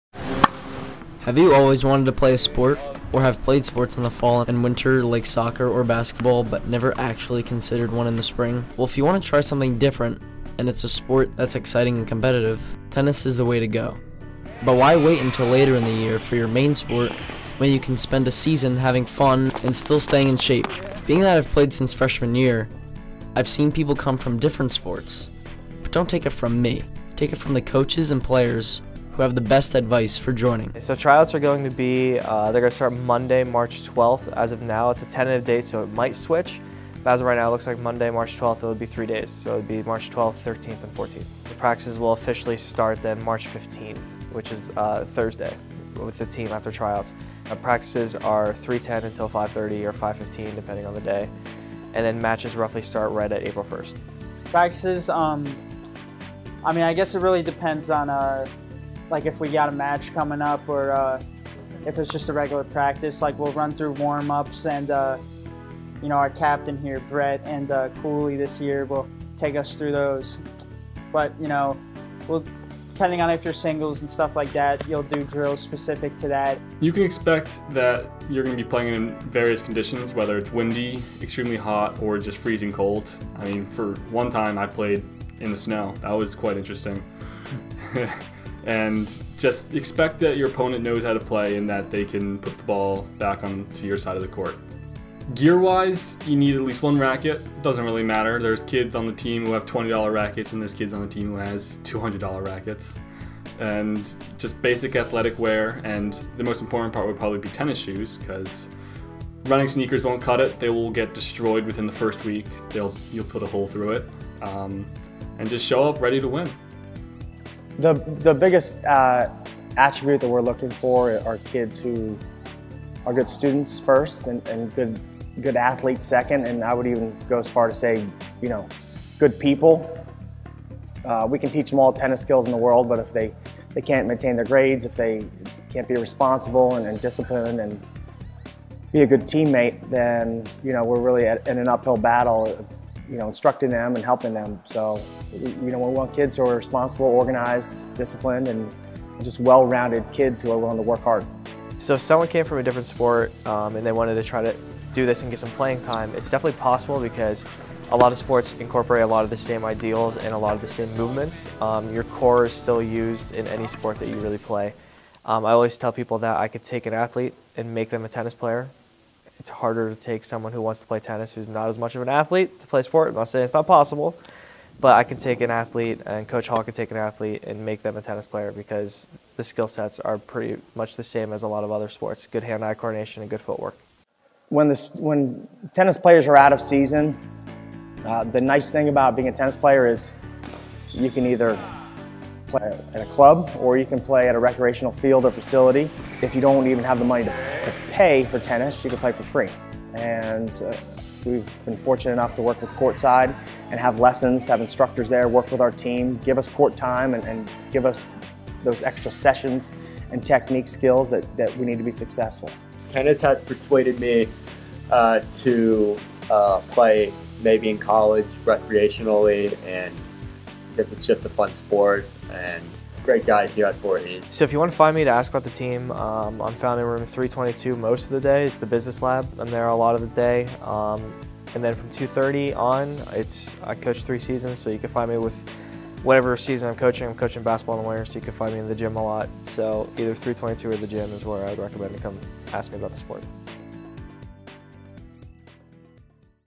Voorhees Tennis Promotion (2017)
Promoted the boys tennis team at Voorhees High School last year. Thought I’d just upload this because why not.